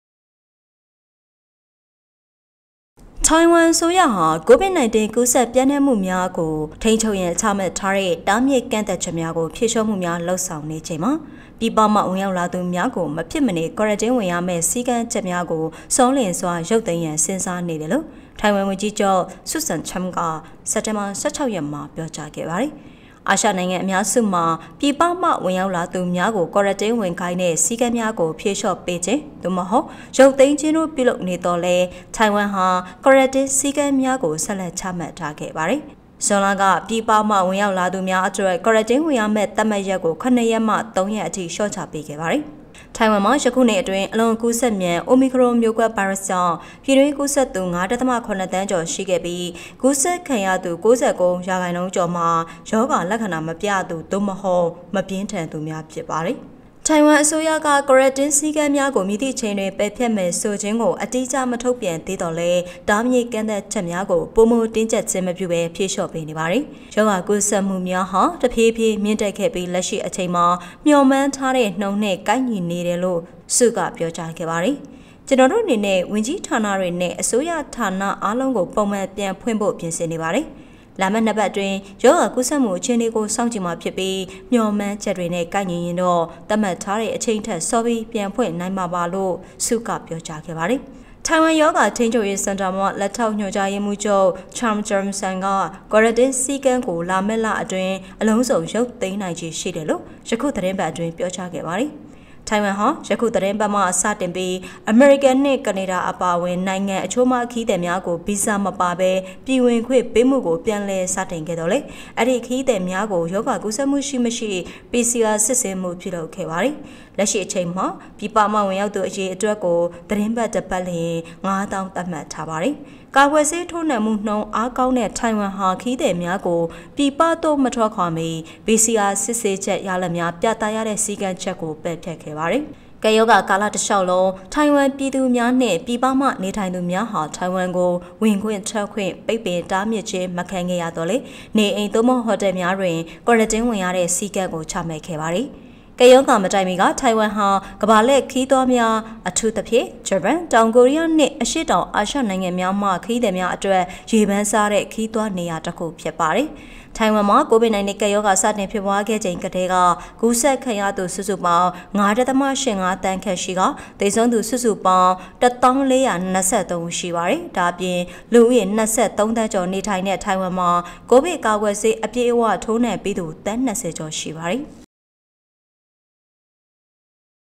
ပြည်ပရောက်လာသူများအတွက် ကွာရန်တင်းဝင်ရမည့် စည်းကမ်းကို ရုပ်သိမ်းရန် စီစဉ်
ပြည်ပရောက်လာသူများအတွက် ကွာရန်တင်းဝင်ရမည့် စည်းကမ်းကို ရုပ်သိမ်းရန် စီစဉ်နေဟု ထိုင်ဝမ် ဝန်ကြီးချုပ်ပြောကြား
 ထိုင်ဝမ် အစိုးရသည် COVID ကူးစက် ပျံနှံမှုများကို ထိန်းချုပ်ရန် ချမှတ်ထားသည့် တားမြစ်ကန့်သတ်ချက်များကို ဖြေလျှော့မှုများ လုပ်ဆောင်နေချိန်တွင် ပြည်ပမှ ဝင်ရောက်လာသူများကို မဖြစ်မနေ ကွာရန်တင်း ဝင်ရမည့် စည်းကမ်းချက်များဆောလျင်စွာရုပ်သိမ်းရန် စဉ်းစားနေတယ်လို့ ထိုင်ဝမ် ဝန်ကြီးချုပ် ဆုဆန်ချန်းက စက်တင်ဘာ ၁၆ ရက်တွင်ပြောကြားပါတယ်၊